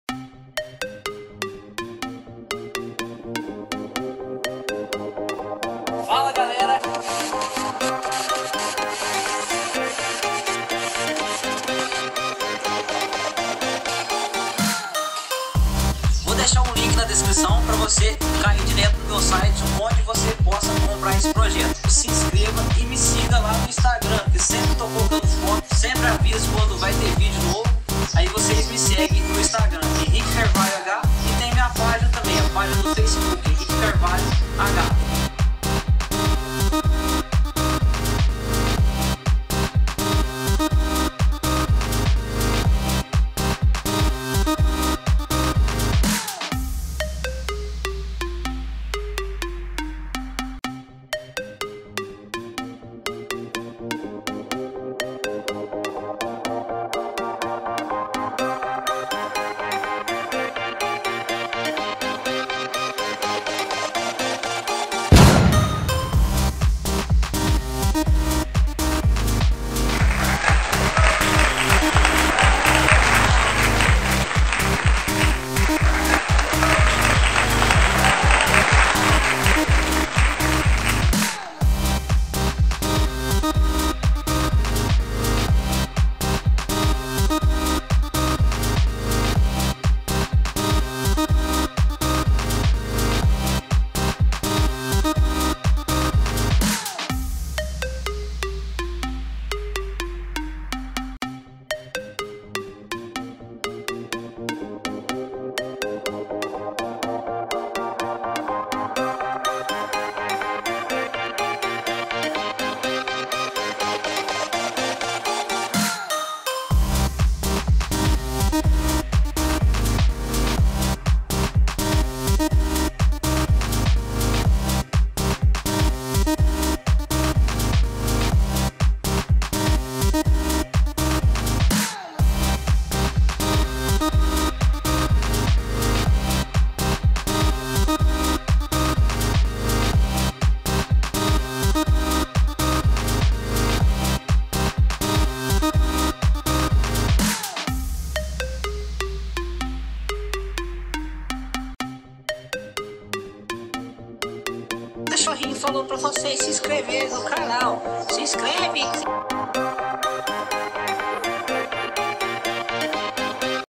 Desbravador - Projeto de Henrique Carvalho 🏎️
Foi finalizado o projeto que muitos não acreditaram que seria possivel mas depois de um pouco mais de 3 anos e meio após muito trabalho, dedicação e inteligência, a equipe HDL (Henrique, Daiane e Lucas) finalizaram a construção do Desbravador (motor de Volvo).